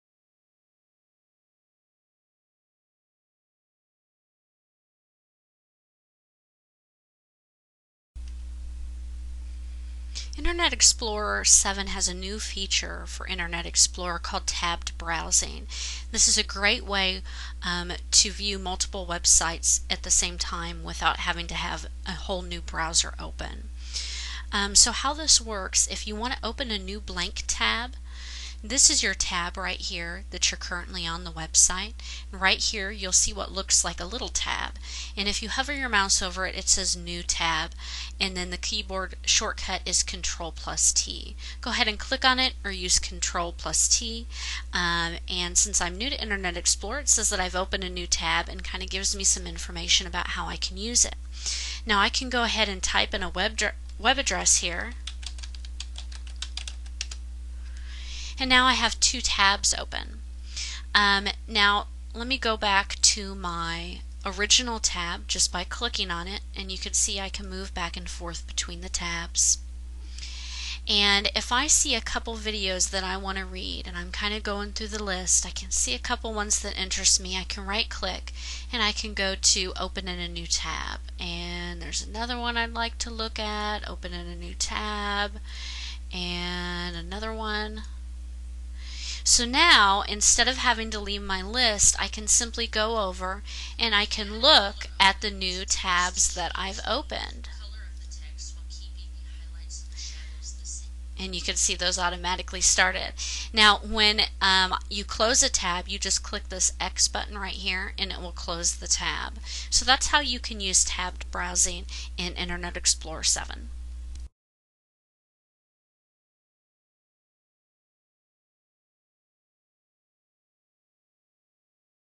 Use Tabs in Internet Explorer 7
See a higher quality video on TechTutor.TV! Tabbed browsing can be used in Internet Explorer 7 to make your online experience even better. Find out in this video how to open tabs, close tabs, open a blank tab and open a tab from a link on a website. Don't delay, check out this video on how to use tabs in Internet Explorer 7. See higher quality tutorials all for FREE